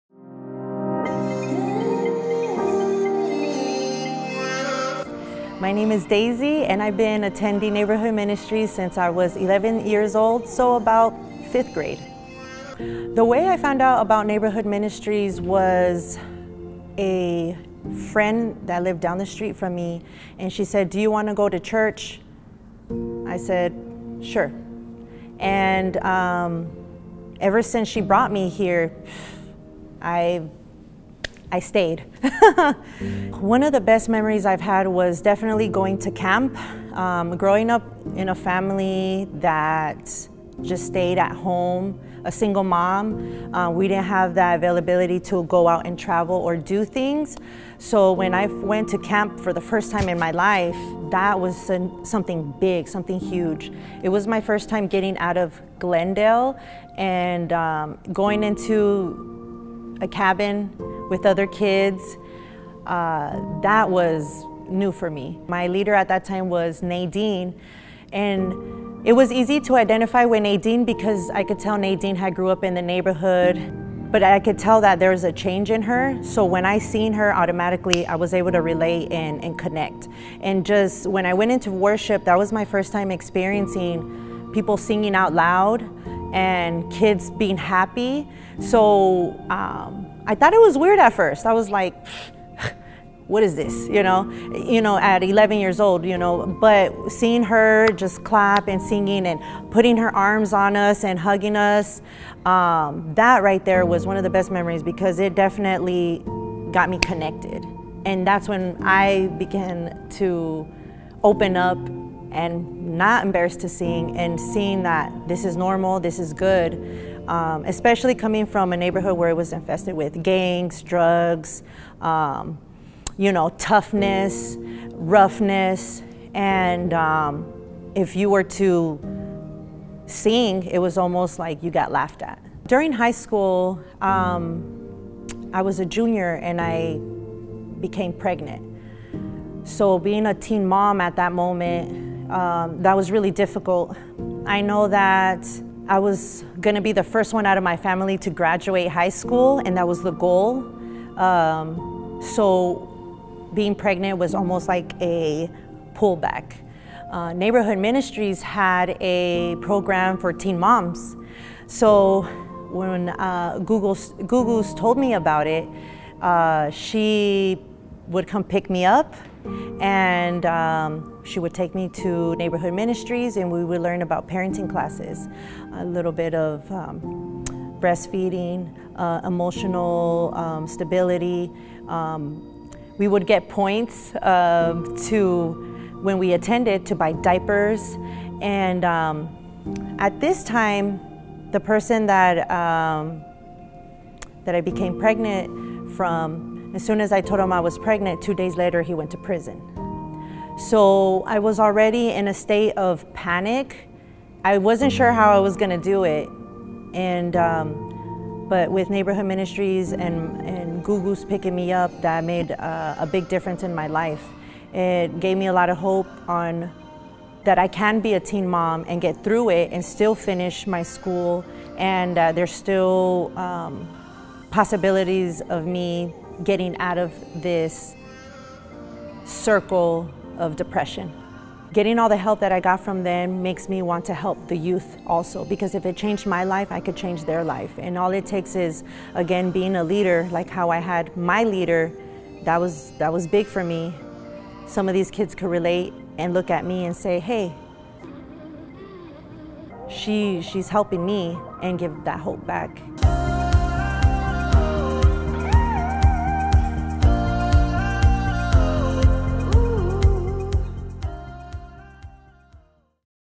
Neighborhood Ministries | Daisy | 40 Stories
Meet Daisy

Her story reminds us that the simple act of an invitation opens up the possibilities of a life transformed. Each person she names in this testimonial was someone who showed up for her, at a particular time in her life, inviting her into the space that changed her. Her neighbor’s invite, Nadine’s invite, Googoos’ invite. We are committed to never, no never, stop inviting the next one into these places of transformation.